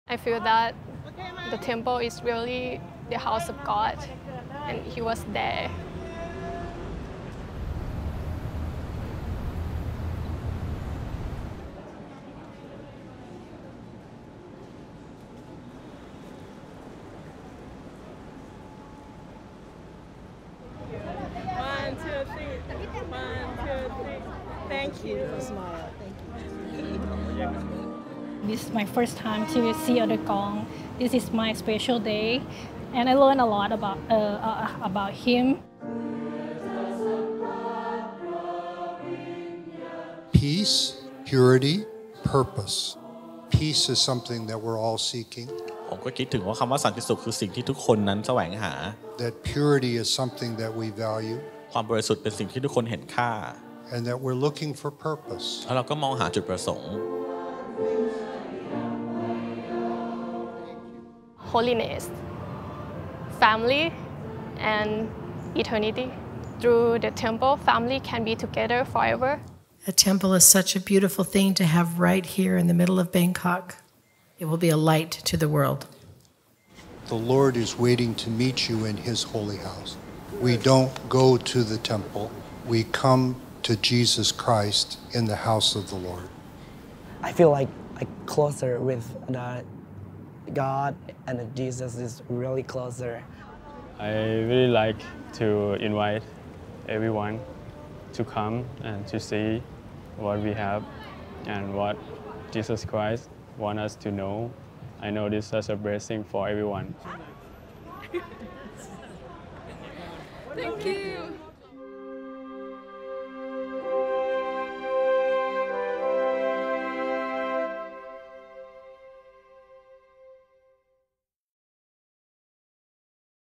Young Single Adults Tour Bangkok Thailand Temple
Traditionally, members of the news media and other invited guests get the first peek at a new house of the Lord. In Bangkok that first look was given to young adults from across Thailand on Saturday, August 26, 2023.

Elder Gerrit W. Gong of the Quorum of the Twelve Apostles and his wife, Susan, greeted each of the more than 375 young adults as they entered the temple.

“This is my first time to see Elder Gong,” said Suchawadee Prasithichai of Bang Khen, a district of Bangkok. “This is my special day. And I learned a lot about him.”

Prior to entering the house of the Lord, the Apostle and his wife held a devotional for the group in a stake center next to the temple.

“Ten years ago, Elder Gong and I were here in Thailand, and we just felt that the blessings of God were ready to be poured out upon the people,” Sister Gong said. “And it’s happening. A temple is such a beautiful thing to have right here in the middle of Bangkok. It will be a light to the world. I am grateful for your goodness and sacrifice which helped make it possible.”